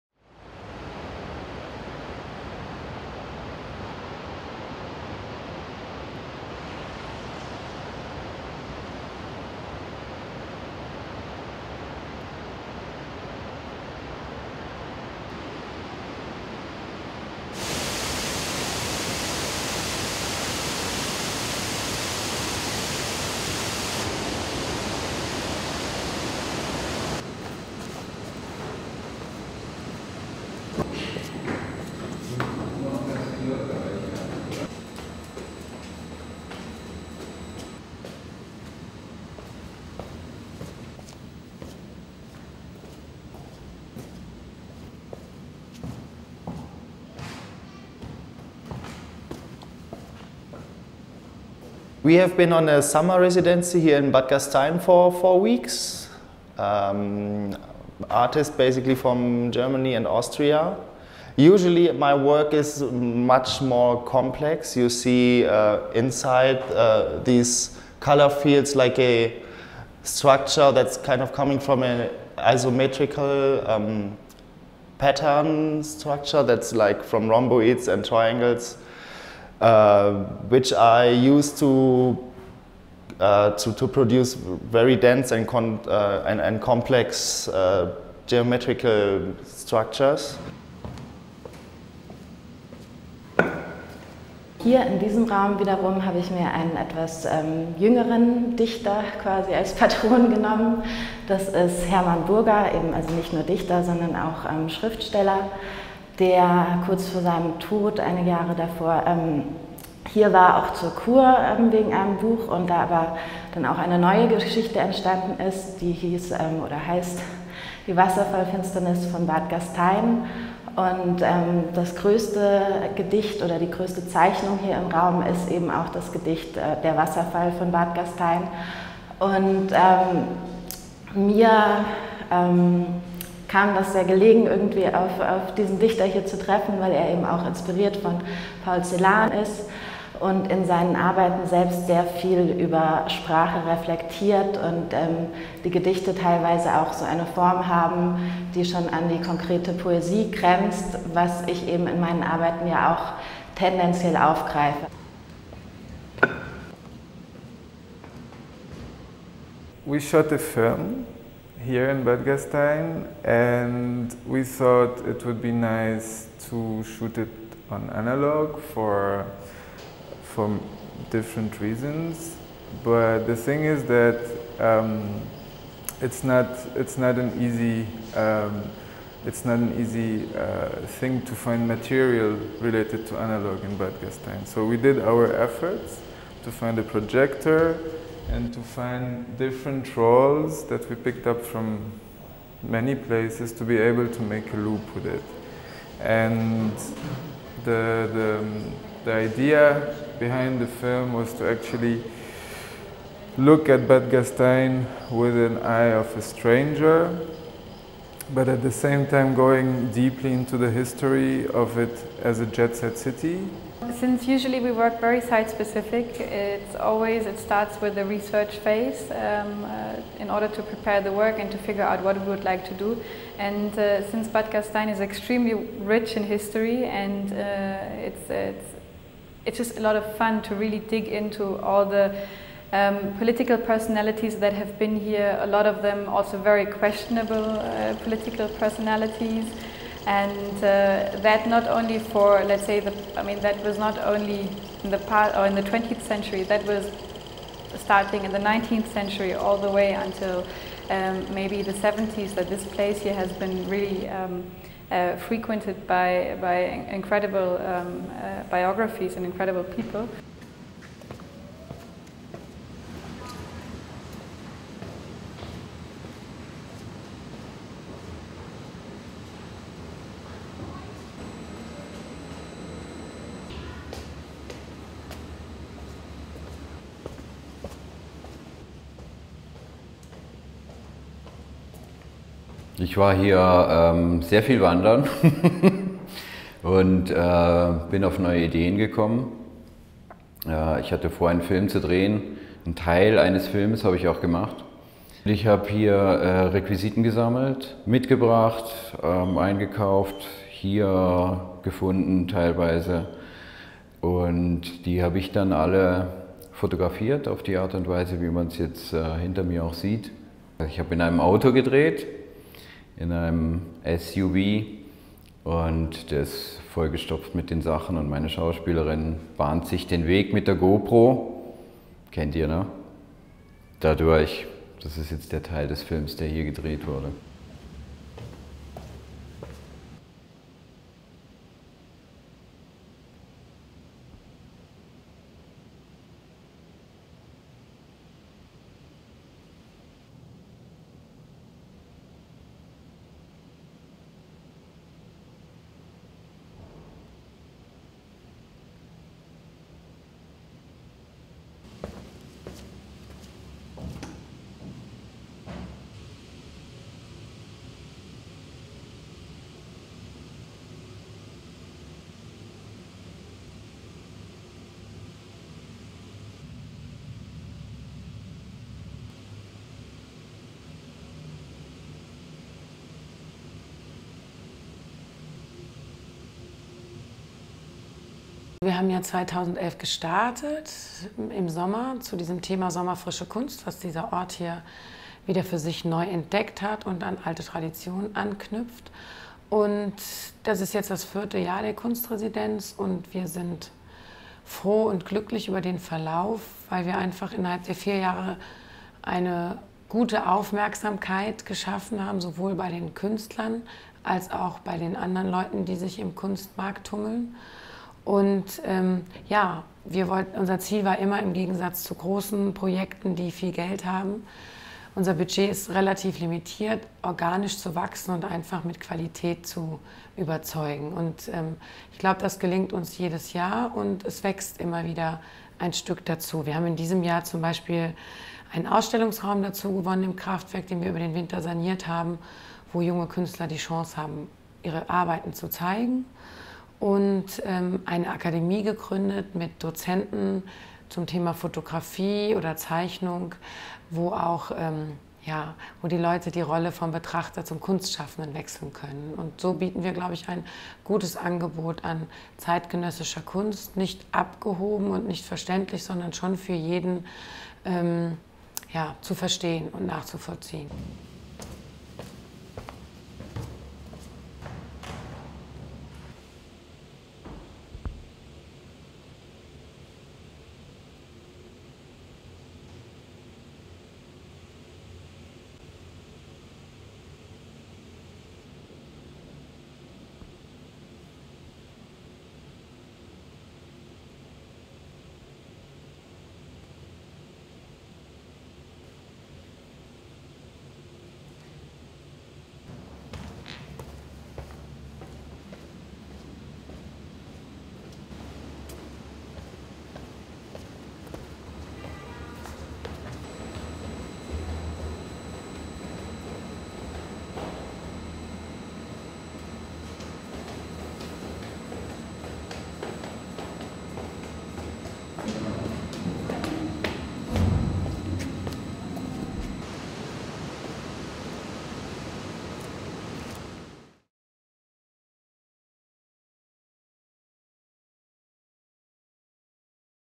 Kunstresidenz Sommer.Frische.Kunst Bad Gastein 2014
Kunstresidenz Sommer.Frische.Kunst Bad Gastein is an Artist-In-Residence program that invites artists to create new work in Bad Gastein, a town in the mountains near Salzburg (Austria). Bad Gastein is a quite unique place to produce art. On the one hand the topography of the city is unique, more vertical than horizontal, dominated by historic multi-story hotel buildings and a waterfall that runs right through the center or the city. And as if this wasn’t enough, there’s also a 70s style Congress Center that looks like a UFO. On the other hand Bad Gastein has a rich history. In the 19th century Bad Gastein became a fashionable resort that was visited by the rich and famous. Today, Bad Gastein offers a rather morbid charm, with many hotels and the Congress Center in the town center vacant. No wonder that many artists get inspired by the unique aura of the place.

On the occasion of the opening of the exhibition that presents the results of the work of the eight artists of this year’s Artist-In-Residence program, we had a look at the show and spoke to some of the artists. We also met with the curator of the program, Andrea von Goetz und Schwanenfliess, who provided us with a look back at 4 years of the Sommer.Frische.Kunst Artist-In-Residence program and an outlook to next year’s anniversary. This year’s artists in residence are Aiko Tezuka, Dirk Meinzer, Franziska Pierwoss, Siska, Michael Conrads, Nick Oberthaler, Stefan Panhans, and Susanne Stroh.

This video is the short version, the full-length video is available on our website.

Kunstresidenz Sommer.Frische.Kunst Bad Gastein 2014 Artist Residency exhibition. Bad Gastein (Austria), July 27, 2014.